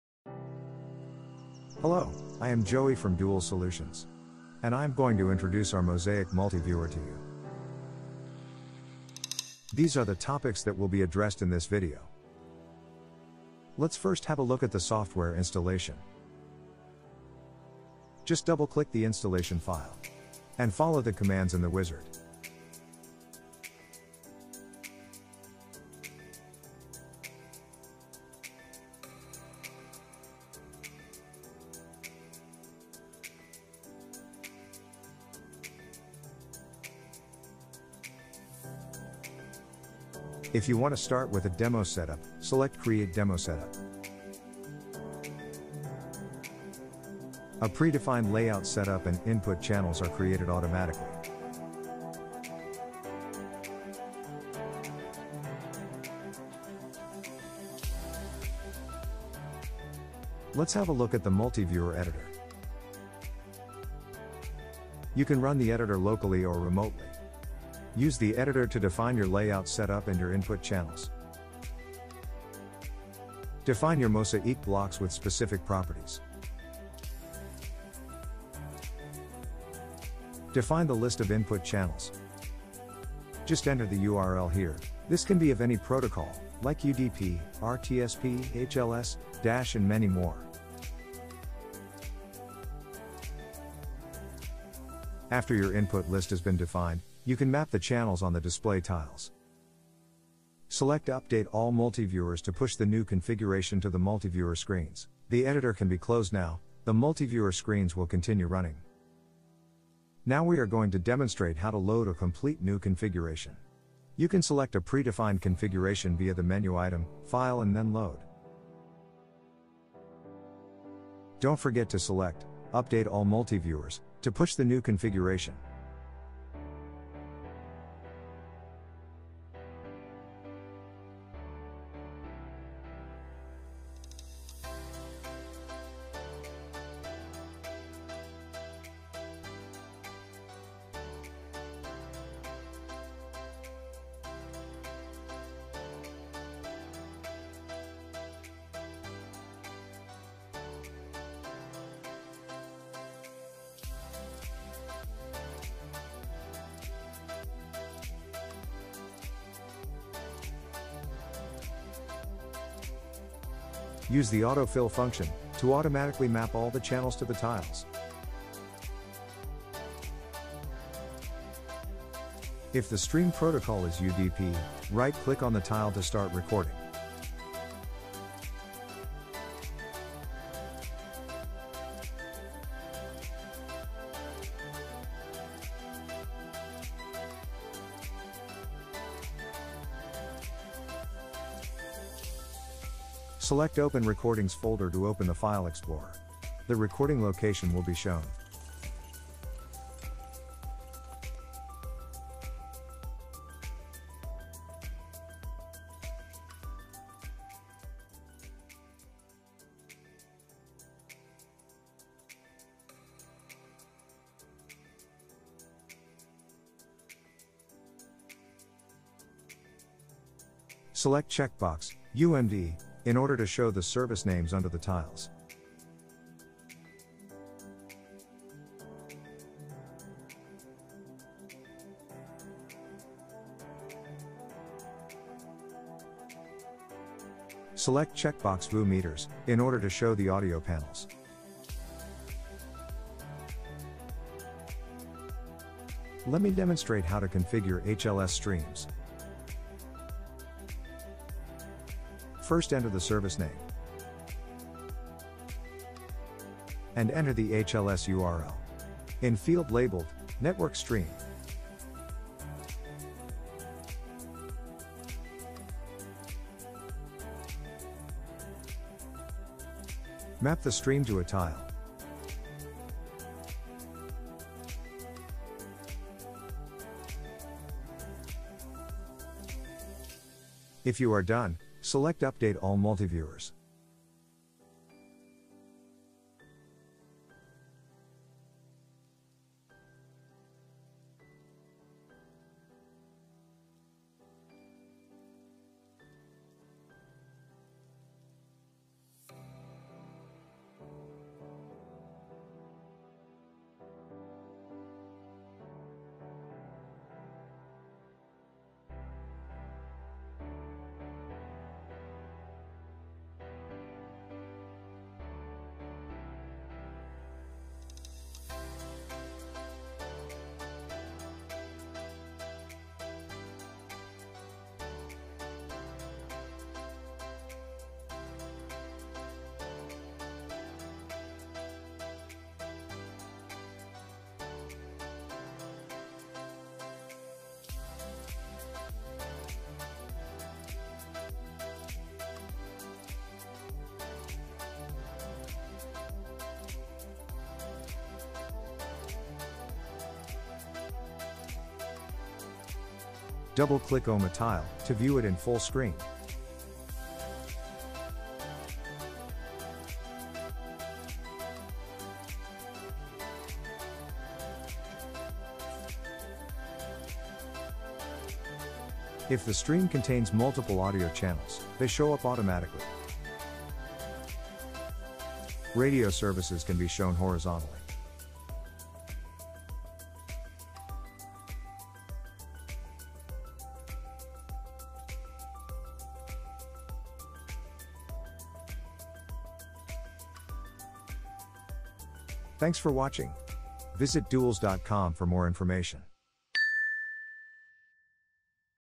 Multiviewer
This video shows all the features of the Dualz MOSAIC IP multiviewer in one overview. The multiview software supports TSoIP transport streams, as well as OTT streams, UDP, RTP, RTSP, RTMP, HLS, DASH, SRT, NDI and much more...
Interfaces with ASI or RF input (DVB-S/S2, DVBT-T2, DVB-C) via Dektec cards. Also support for HDMI and HD-SDI inputs.

Would you like to have all your channels at a glance, supporting TS over IP, UDP, SRT, RTSP, RTMP, RTP, and a full overview of your video services? Then the Mosaic IP Multiviewer is the solution for you. SRT, NDI, HLS, DASH, HDMI/SDI (optional), and much more. The system features functions such as layout presets, round robin and penalty box, giving a visual overview of all channels. It shows an alarm indication in case audio or video is lost.

MOSAIC EDITOR
The central multiviewer editor configures all your video services over multiple screens in the network. Configure features like round robin mode, HW acceleration, Under Monitor Display, alarm notification and more. All from remote, in your company network or
over internet.

ALARM NOTIFICATION
MOSAIC now reports video loss and audio loss to a central web server. Just configure the web url of the web server (included in software package). Open the web client on the same system or from remote. If you need more analysis and 24×7 alarm monitoring, have a look at our SMART solutions.

HARDWARE SUGGESTION
SuperMicro 1U 5019C-WR E2200 X11
Intel 6C E2286G 4.0G 12M 95W P630
1x SSD Samsung 240 GB PM883
1x 16 GB DDR4 2666MHZ RAM ECC
Graphics/GPU Nvidia 1050 TI 4GB
Windows 10 64 bit Professional UK version

Streaming multiviewer output over UDP.
Also supports recording of TS samples.
Available in software-only (lite) or appliance.